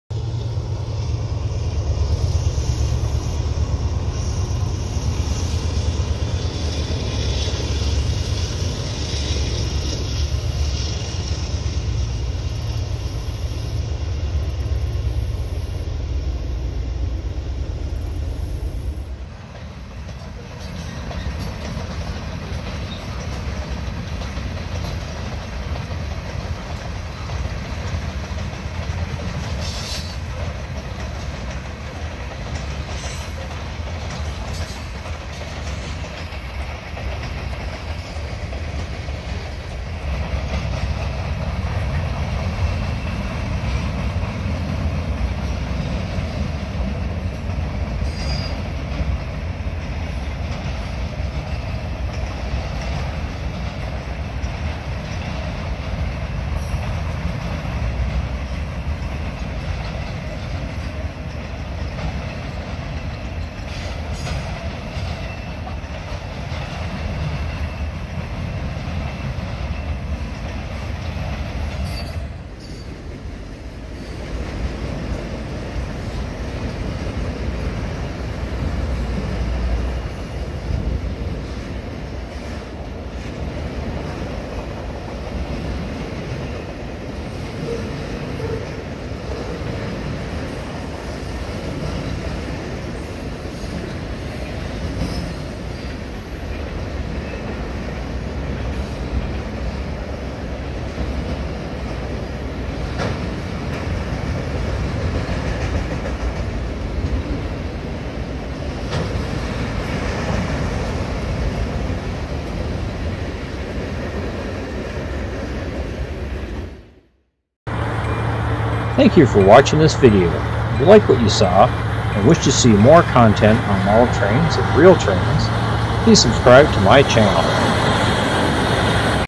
RAILFANNING: CP MILTON AUGUST 26 2023
I managed to just catch an eastbound mixed freight.
The freight is led by:
AC4400CW (9820) & AC4400CWM rebuild (8057)